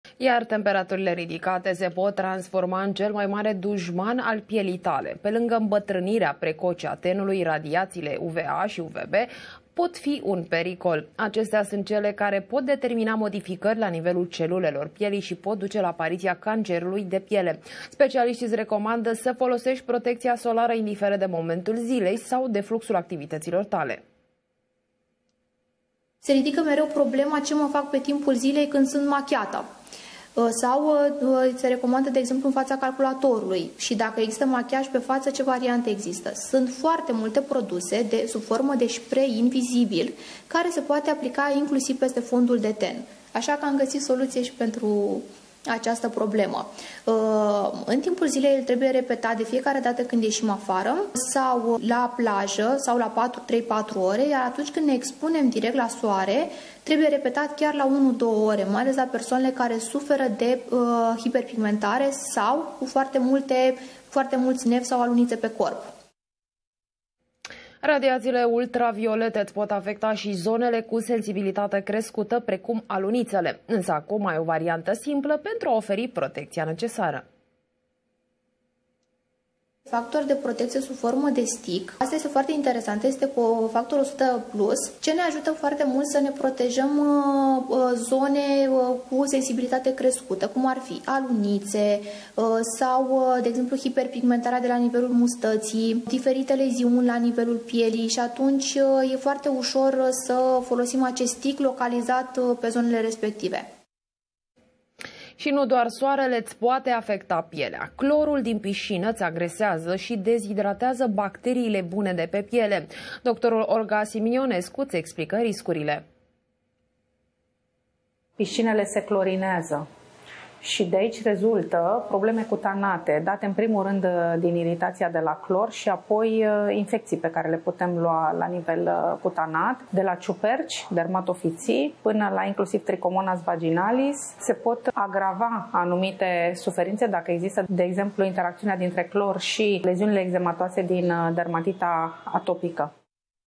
ȘTII: Cum să-ți protejezi pielea pe timp de vară?
➥ Temperaturile ridicate se pot transforma în cel mai mare dușman al pielii tale.
➥ Radiațiile UVA și UVB pot determina modificări la nivelul celulelor.
➥ Specialiștii recomandă folosirea cremei solare indiferent de momentul zilei.